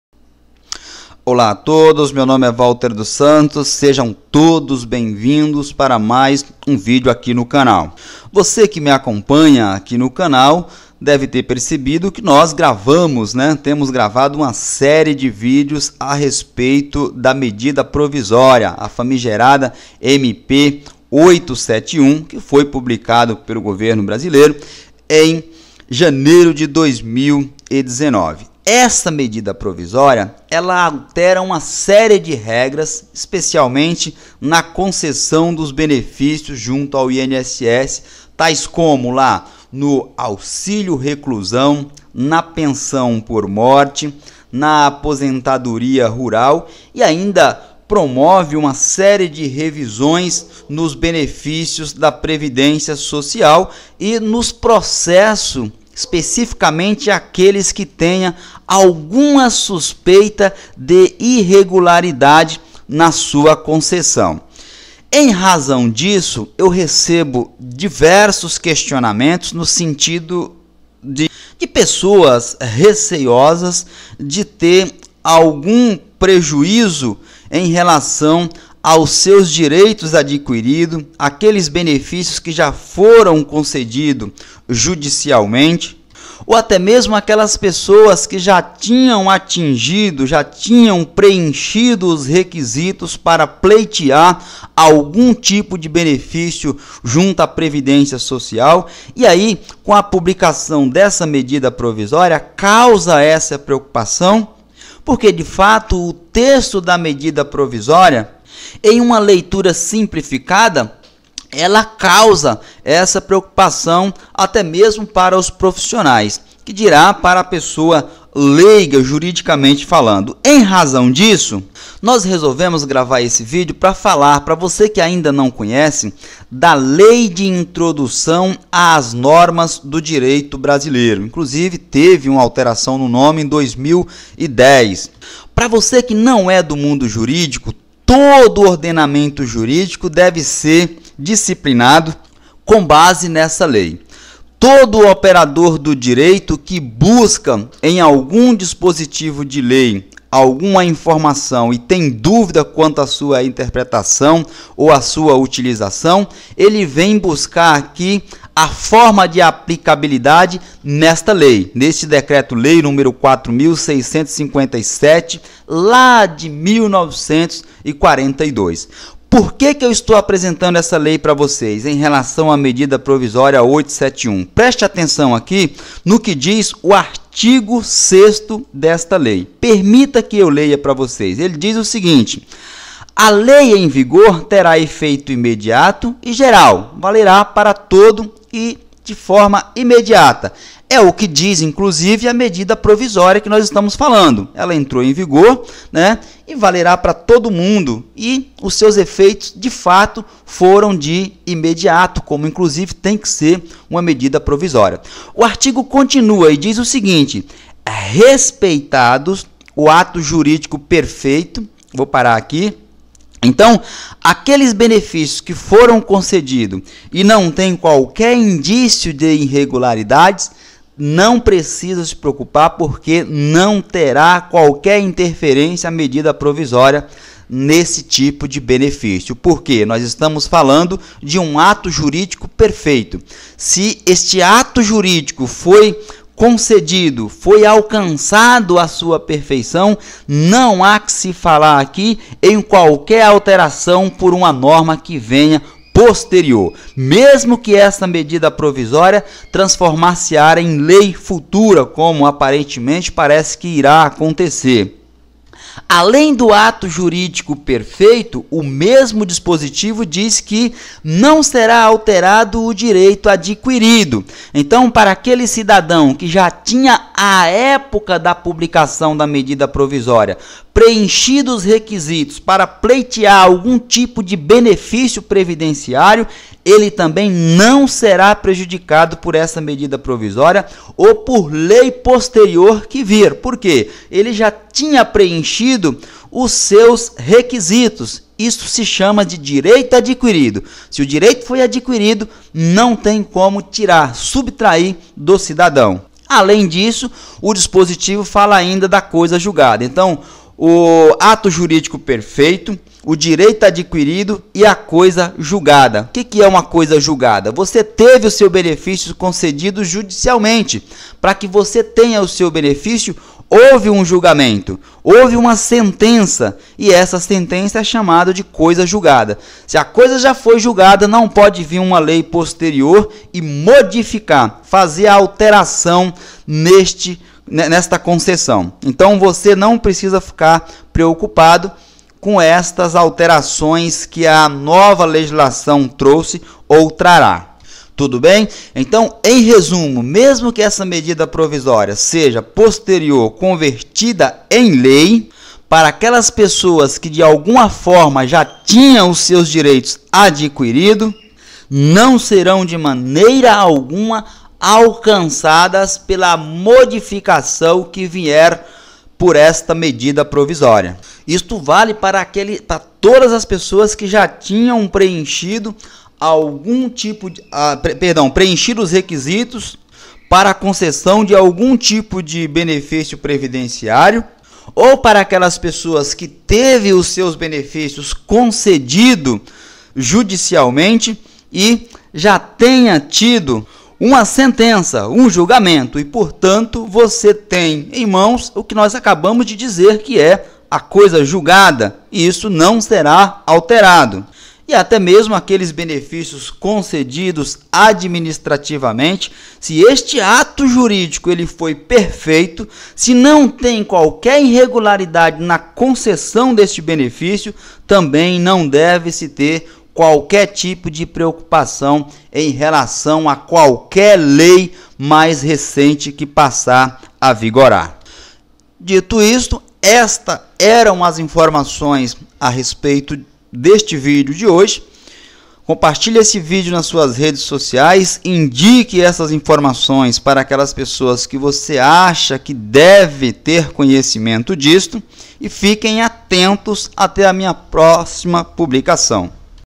Direito adquiridos aos benefícios do INSS e a Lei posterior
Medida Provisória (MP) nº 871, de 18 de janeiro de 2019
...altera regras de concessão de benefícios, como auxílio-reclusão, pensão por morte e aposentadoria rural, e promove revisão de benefícios e de processos com suspeitas de irregularidades no Instituto Nacional do Seguro Social (INSS).